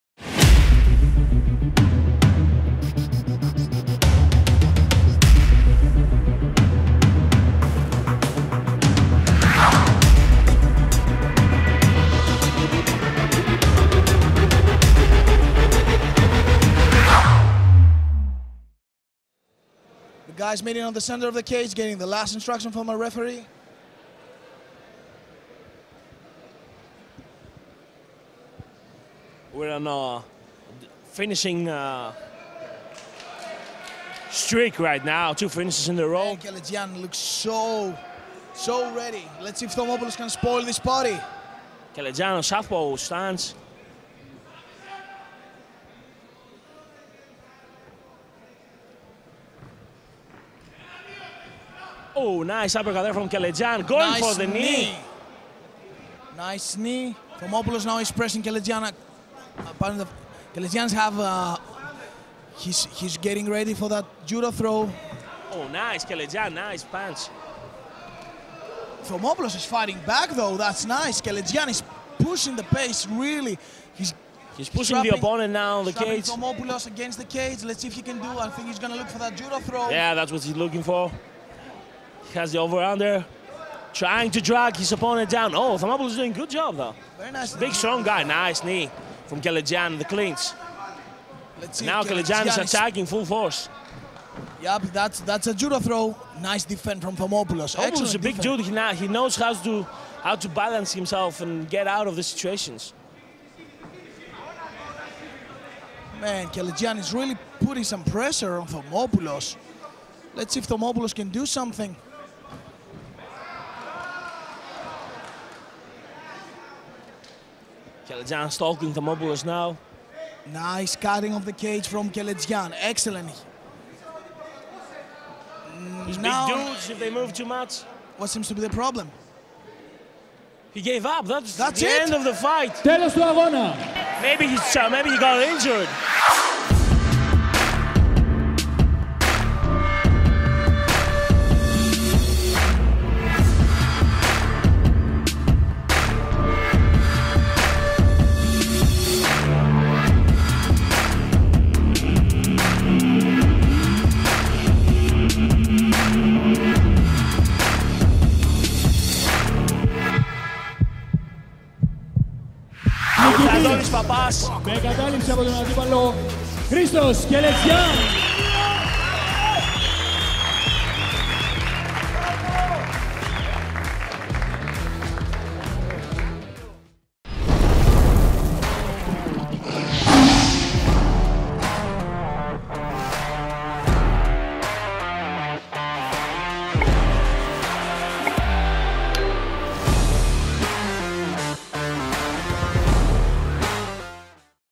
Cage Survivor 5 | Kostas Thomopoulos vs. Christos Kelletzian | Full Fight
Name of Promotion: Cage Survivor
Name of Event: Cage Survivor 5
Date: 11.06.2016
Place: D.A.K. Glifadas
City: Athens
Country: Greece
Kostas Thomopoulos (0-0-0, Greece) vs. Christos Kelletzian (1-0-0, Greece)

Chapters:
0:00​​​  Intro
0:20  1st Round
2:43  Winner is...